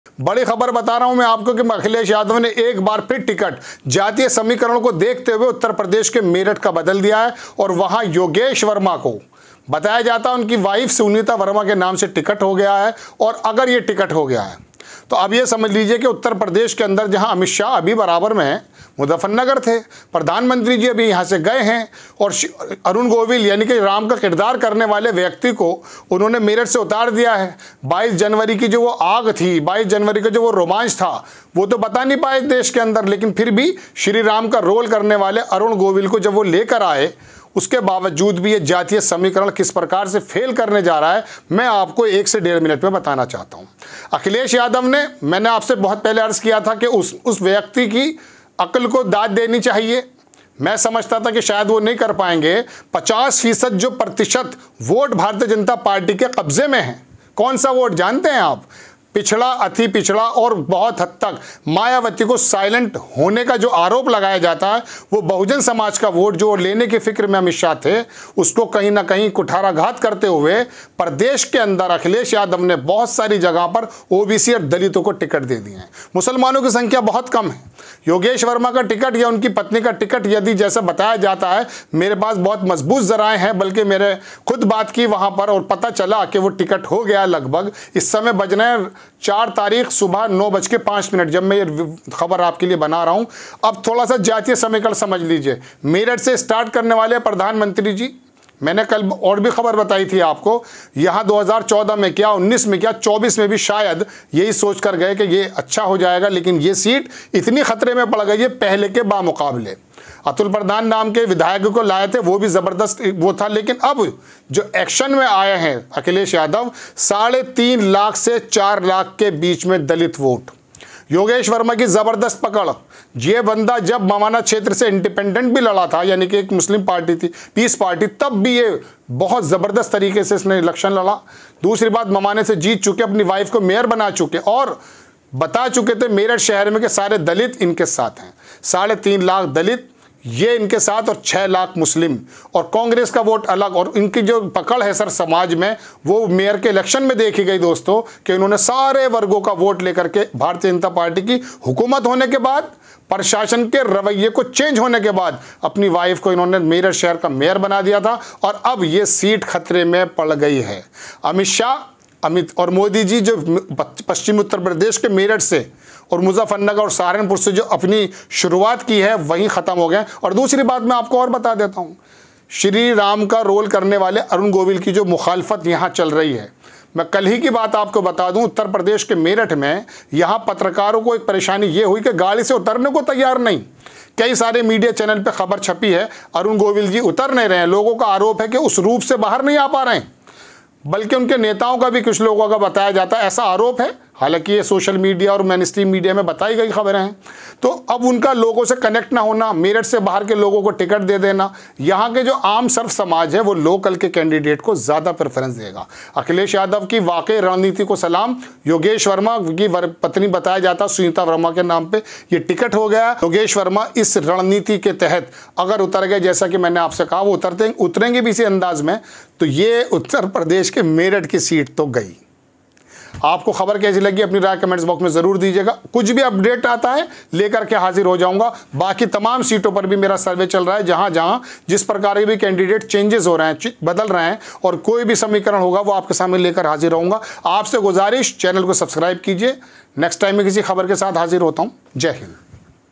मेरठ से अतुल प्रधान का टिकट कटायोगेश वर्मा की पत्नी का टिकट फाइनल news nine tv
मेरठ से अतुल प्रधान का टिकट कटा

योगेश वर्मा की पत्नी का टिकट फाइनल  news nine tv

SP Meerut Candidate: सपा ने मेरठ सीट पर बदला उम्मीदवार | Meerut | Atul Pradhan | Akhilesh Yadav
news nine tv
समाजवादी पार्टी ने मेरठ से विधायक अतुल प्रधान को लोकसभा का प्रत्याशी बनाया. भानु प्रताप का टिकट कटा. मेरठ के सरधना से समाजवादी पार्टी के विधायक हैं अतुल प्रधान.